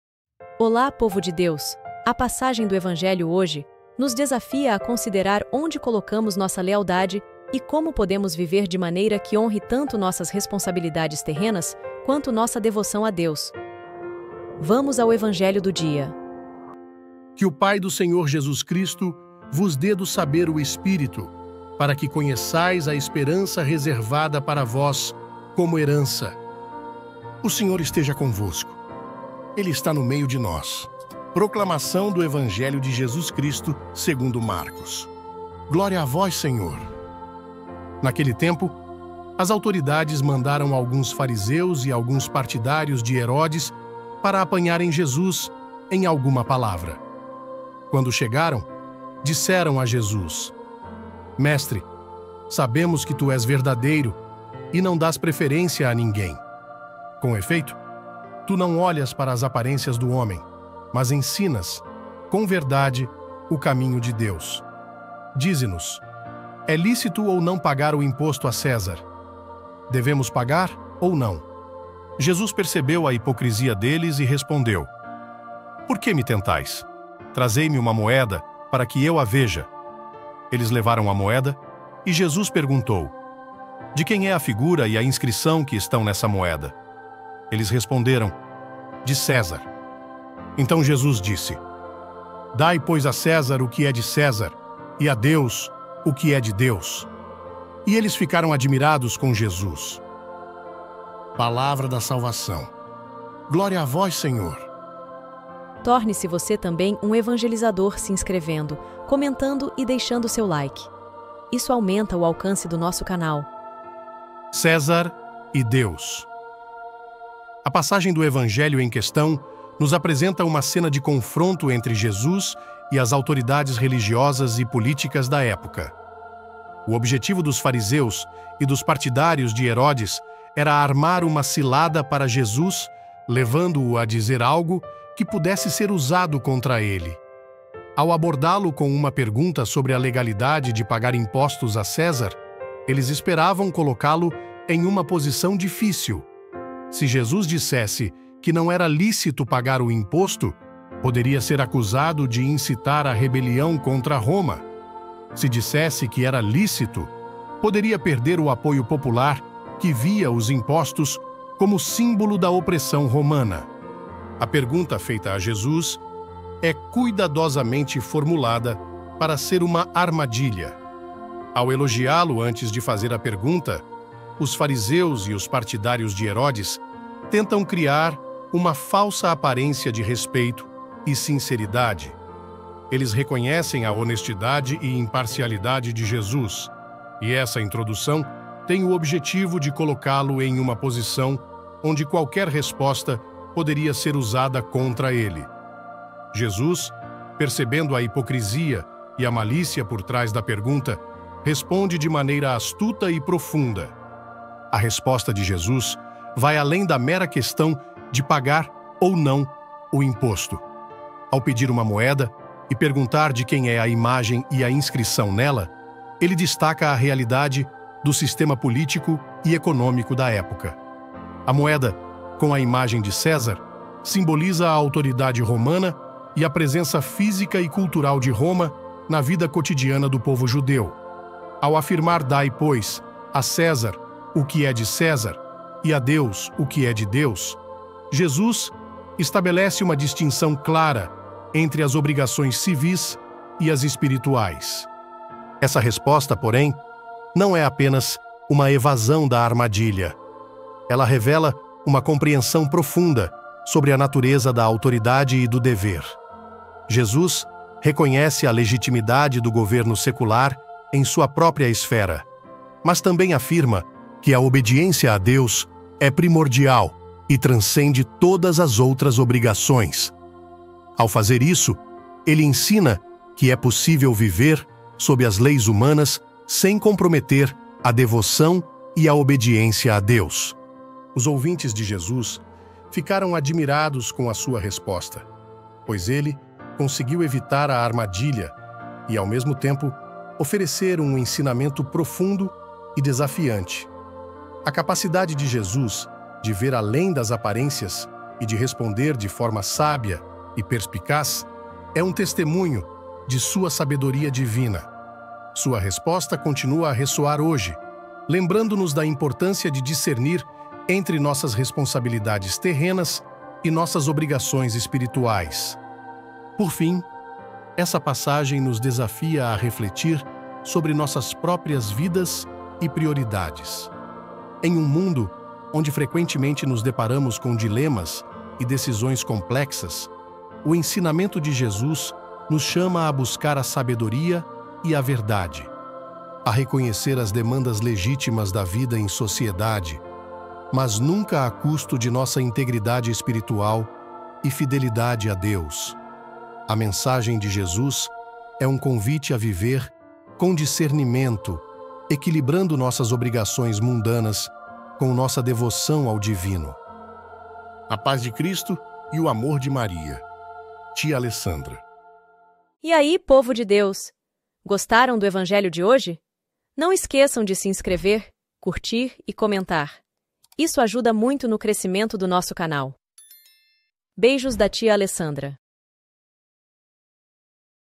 EVANGELHO DO DIA 04/06/2024 | Reflexão e Evangelho de Hoje | César e Deus | Marcos 12,13-17
LITURGIA DIÁRIA: 9ª Semana do Tempo Comum | Terça-feira
- EVANGELHO DE HOJE – Marcos 12,13-17
— Que o Pai do Senhor Jesus Cristo vos dê do saber o Espírito, para que conheçais a esperança reservada para vós como herança! (Ef 1,17s)
Naquele tempo, as autoridades mandaram alguns fariseus e alguns partidários de Herodes para apanharem Jesus em alguma palavra. Quando chegaram, disseram a Jesus: “Mestre, sabemos que tu és verdadeiro e não dás preferência a ninguém. Com efeito, tu não olhas para as aparências do homem, mas ensinas, com verdade, o caminho de Deus. Dize-nos: é lícito ou não pagar o imposto a César? Devemos pagar ou não?”
Jesus percebeu a hipocrisia deles e respondeu: “Por que me tentais? Trazei-me uma moeda para que eu a veja”. Eles levaram a moeda, e Jesus perguntou: “De quem é a figura e a inscrição que estão nessa moeda?” Eles responderam: “De César”. Então Jesus disse: “Dai, pois, a César o que é de César e a Deus o que é de Deus”. E eles ficaram admirados com Jesus.

Reflexão do Evangelho
- César e Deus.
A passagem do Evangelho em questão nos apresenta uma cena de confronto entre Jesus e as autoridades religiosas e políticas da época. O objetivo dos fariseus e dos partidários de Herodes era armar uma cilada para Jesus, levando-o a dizer algo que pudesse ser usado contra Ele. Ao abordá-lo com uma pergunta sobre a legalidade de pagar impostos a César, eles esperavam colocá-lo em uma posição difícil: se Jesus dissesse que não era lícito pagar o imposto, poderia ser acusado de incitar a rebelião contra Roma; se dissesse que era lícito, poderia perder o apoio popular que via os impostos como símbolo da opressão romana.
A pergunta feita a Jesus é cuidadosamente formulada para ser uma armadilha. Ao elogiá-lo antes de fazer a pergunta, os fariseus e os partidários de Herodes tentam criar uma falsa aparência de respeito e sinceridade. Eles reconhecem a honestidade e imparcialidade de Jesus, e essa introdução tem o objetivo de colocá-lo em uma posição onde qualquer resposta poderia ser usada contra Ele. Jesus, percebendo a hipocrisia e a malícia por trás da pergunta, responde de maneira astuta e profunda.
A resposta de Jesus vai além da mera questão de pagar ou não o imposto. Ao pedir uma moeda e perguntar de quem é a imagem e a inscrição nela, Ele destaca a realidade do sistema político e econômico da época. A moeda, com a imagem de César, simboliza a autoridade romana e a presença física e cultural de Roma na vida cotidiana do povo judeu. Ao afirmar “Dai, pois, a César o que é de César e a Deus o que é de Deus”, Jesus estabelece uma distinção clara entre as obrigações civis e as espirituais.
Essa resposta, porém, não é apenas uma evasão da armadilha. Ela revela uma compreensão profunda sobre a natureza da autoridade e do dever. Jesus reconhece a legitimidade do governo secular em sua própria esfera, mas também afirma que a obediência a Deus é primordial e transcende todas as outras obrigações. Ao fazer isso, Ele ensina que é possível viver sob as leis humanas sem comprometer a devoção e a obediência a Deus.
Os ouvintes de Jesus ficaram admirados com a sua resposta, pois Ele conseguiu evitar a armadilha e, ao mesmo tempo, oferecer um ensinamento profundo e desafiante. A capacidade de Jesus de ver além das aparências e de responder de forma sábia e perspicaz é um testemunho de Sua sabedoria divina. Sua resposta continua a ressoar hoje, lembrando-nos da importância de discernir entre nossas responsabilidades terrenas e nossas obrigações espirituais.
Por fim, essa passagem nos desafia a refletir sobre nossas próprias vidas e prioridades. Em um mundo onde frequentemente nos deparamos com dilemas e decisões complexas, o ensinamento de Jesus nos chama a buscar a sabedoria e a verdade, a reconhecer as demandas legítimas da vida em sociedade, mas nunca a custo de nossa integridade espiritual e fidelidade a Deus. A mensagem de Jesus é um convite a viver com discernimento, equilibrando nossas obrigações mundanas com nossa devoção ao divino.
Tia Alessandra